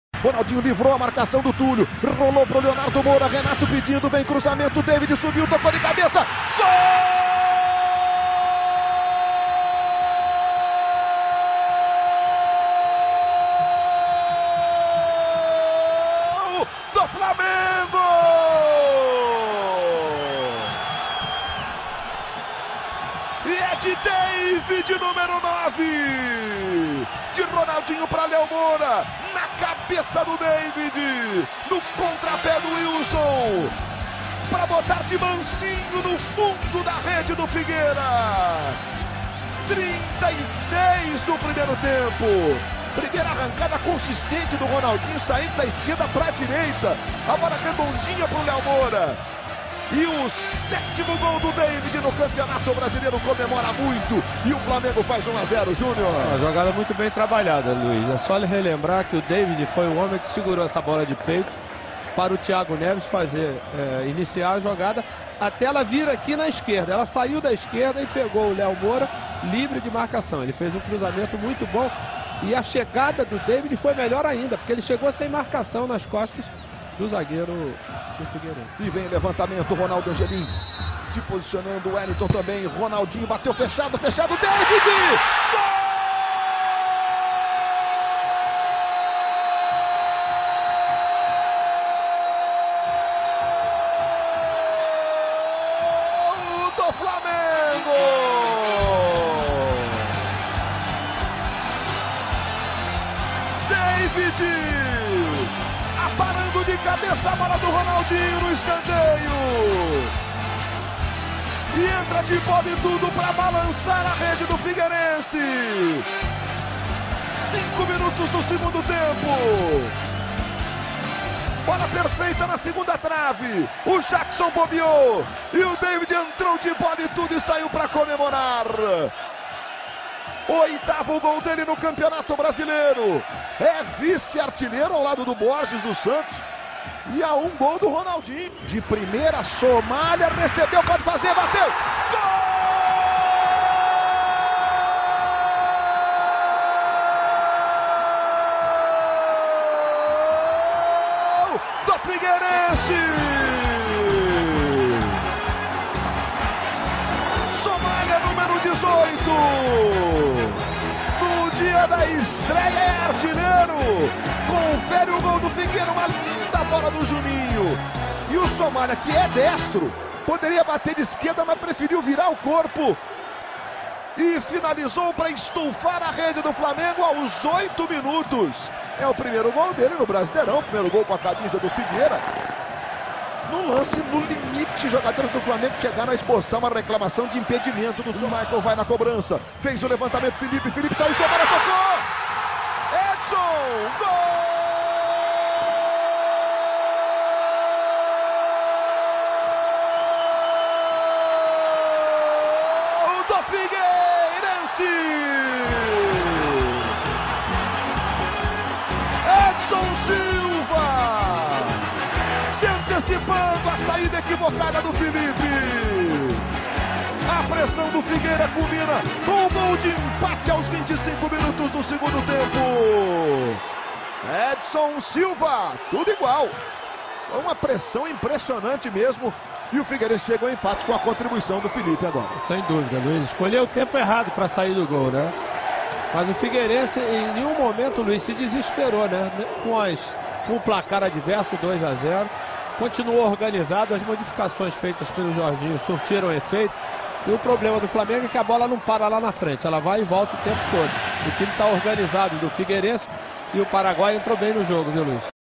Figueirense 2 x 2 Flamengo - Brasileirão 2011
16a rodada - 14/08/2011

O Flamengo vencia o Figueirense por 2 a 0 até os oito minutos do segundo tempo graças à combinação que o levou às primeiras colocações do Brasileirão 2011: uma defesa segura e o talento de Ronaldinho Gaúcho na frente. No entanto, depois de abrir vantagem com dois gols de Deivid e participação decisiva do R10, o Fla não suportou a pressão do organizado time catarinense no Orlando Scarpelli e cedeu o empate por 2 a 2, em duelo pela 16ª rodada.